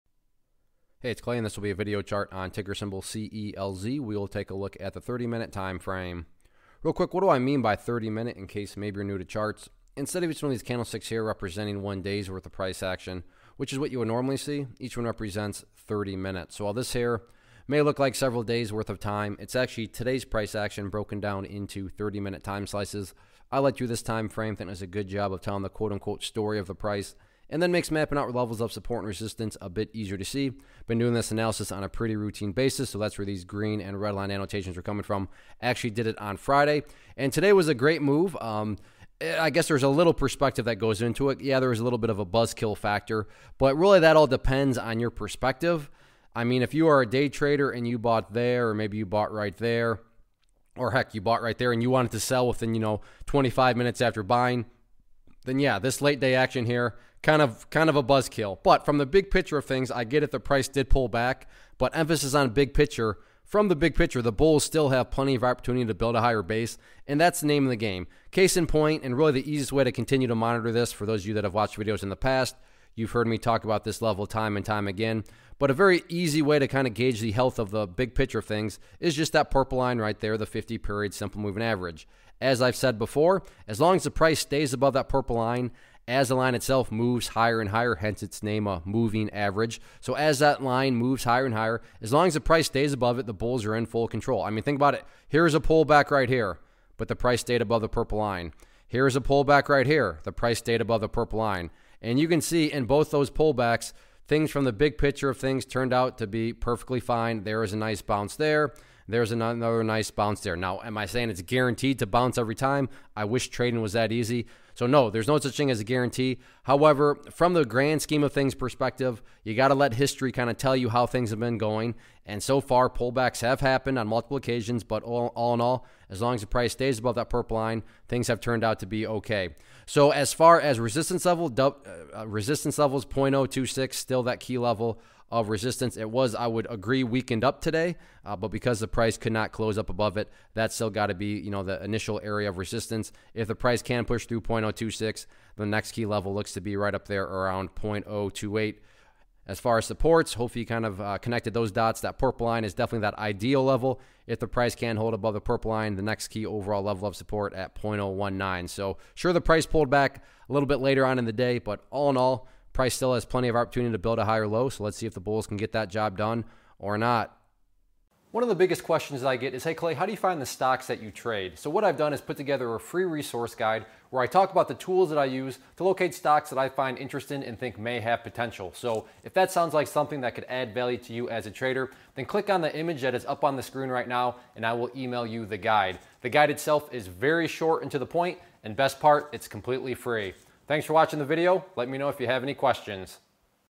Creative Medical Technology Holdings, Inc. - CELZ Stock Chart Technical Analysis for 04-30-18
Learn how to read stock charts and identify technical patterns as ClayTrader does a quick stock chart review on Creative Medical Technology Holdings, Inc. (CELZ).